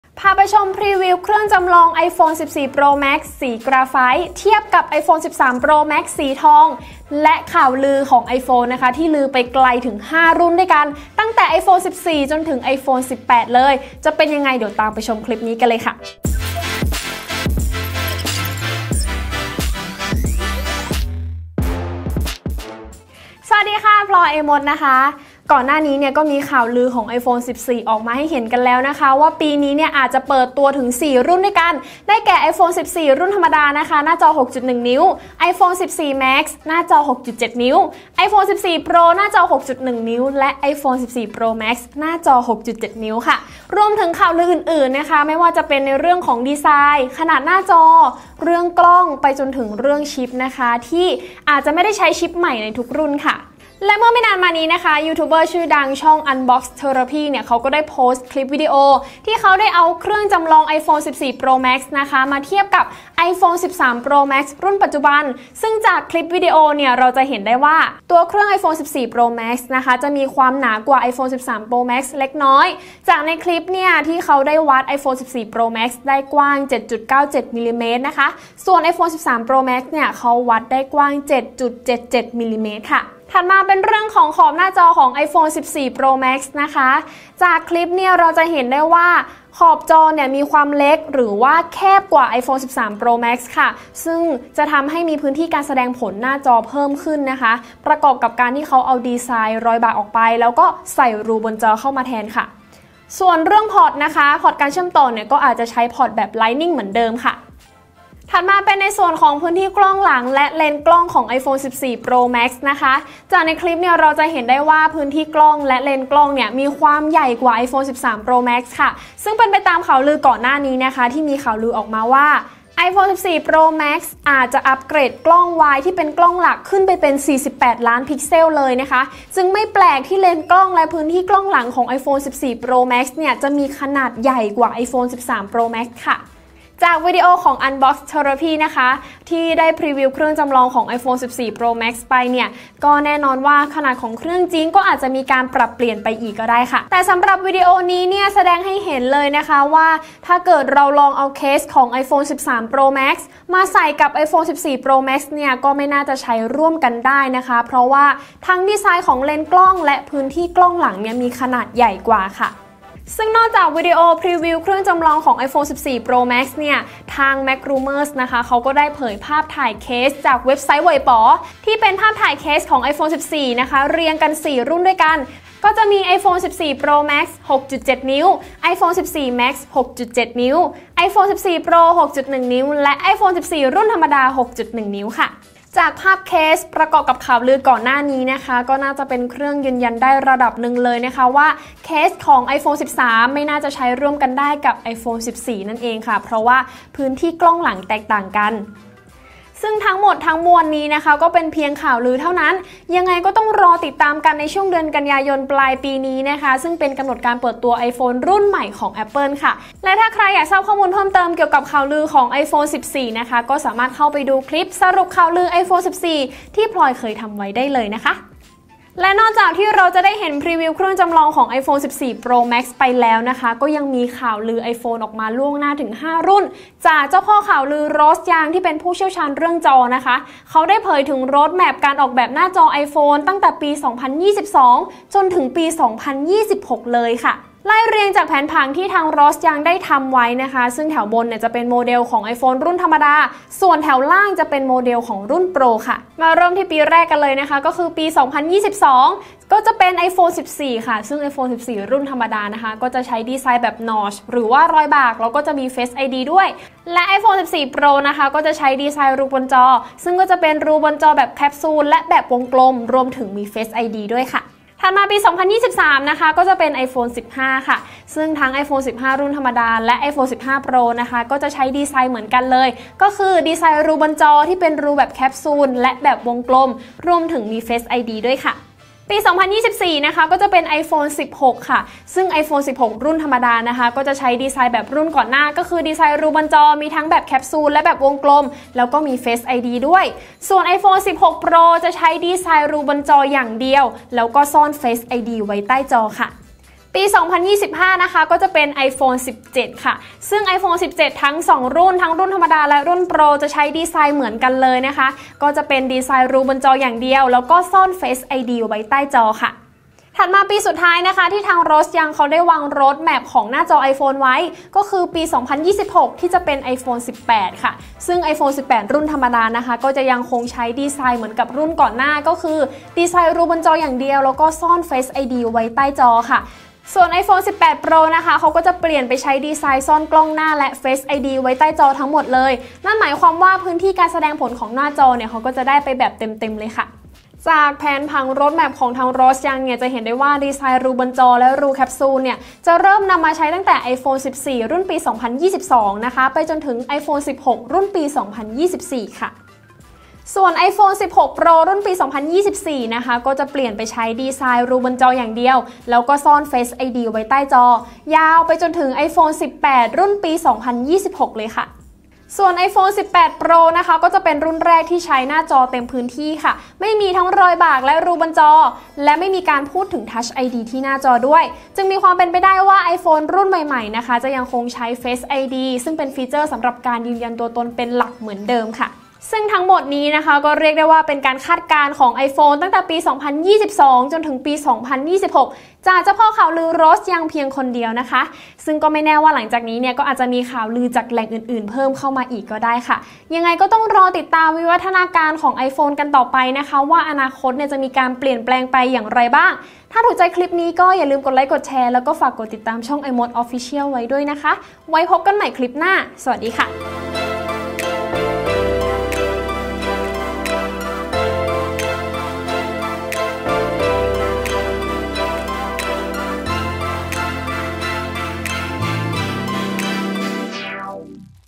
ลือข้าม 5 ปี iPhone 18 Pro ไม่มีรูบนจอ, เผยโมเดลเหมือนจริง iPhone 14 Pro Max | iMoD
นอกจากข่าวลือ iPhone 14 แล้วก็มีรายงานแผนการออกแบบหน้าจอ iPhone ว่า iPhone 8 อีก 5 ปีข้างหน้าจะไม่มีรอยบากและรูบนจอ! ส่วน iPhone 14 ก็เริ่มมีการพรีวิวโมเดลจำลองเหมือนจริงมาให้ชมกันแล้ว

00:00 - Intro
00:23 - iPhone 14 อาจเปิดตัว 4 รุ่น
00:53 - วิดีโอพรีวิวเครื่องจำลอง iPhone 14 Pro Max
01:05 - ความหนาของตัวเครื่อง iPhone 14 Pro Max
01:23 - ขอบหน้าจอของตัวเครื่อง iPhone 14 Pro Max
01:47 - พื้นที่กล้องหลังและเลนส์กล้องของตัวเครื่อง iPhone 14 Pro Max
02:16 - ทดลองใส่เคส iPhone 13 ใน iPhone 14
03:38 - Roadmap ดีไซน์หน้าจอ iPhone ปี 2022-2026
04:06 - (ลือ) หน้าจอ iPhone 14 รุ่นปี 2022
04:24 - (ลือ) หน้าจอ iPhone 15 รุ่นปี 2023
04:39 - (ลือ) หน้าจอ iPhone 16 รุ่นปี 2024
04:56 - (ลือ) หน้าจอ iPhone 17 รุ่นปี 2025
05:09 - (ลือ) หน้าจอ iPhone 18 รุ่นปี 2026
05:37 - สรุป

ℹ️ ข้อมูลเพิ่มเติม :

⋅ เผยภาพถ่ายเคส iPhone 14 ทั้ง 4 รุ่น
⋅ Unbox Therapy พรีวิวเครื่องจำลอง iPhone 14 Pro Max
⋅ Ross Young เผย Roadmap ดีไซน์หน้าจอ iPhone ปี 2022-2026

🔴 ชมคลิปมัดรวมข่าวลือ iPhone 14